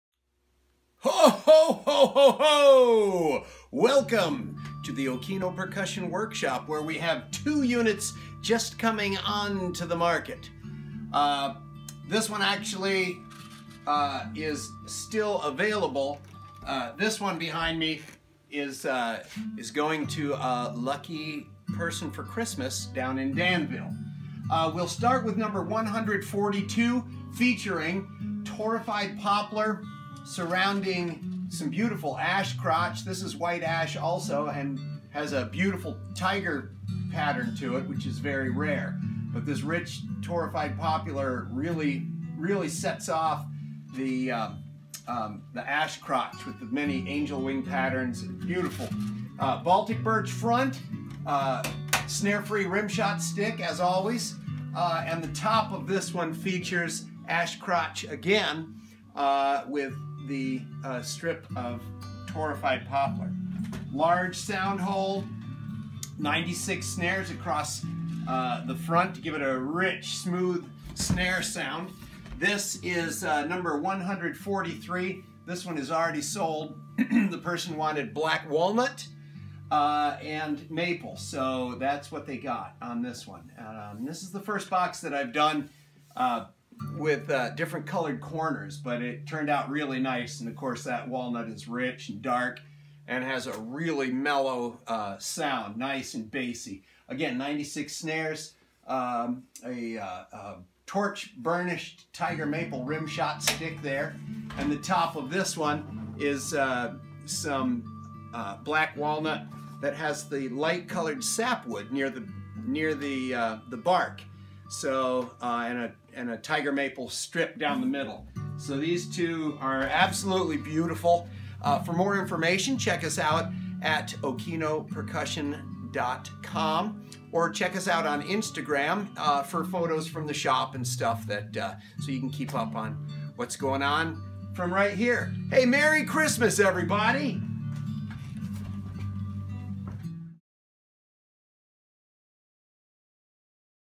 Occhino Percussion Cajon #'s 142 & 143
Two beautiful solid wood cajon drums, just completed before Christmas.  One featuring Torrefied Poplar and White Ash Crotch, and the other featuring Black Walnut with Hard Maple.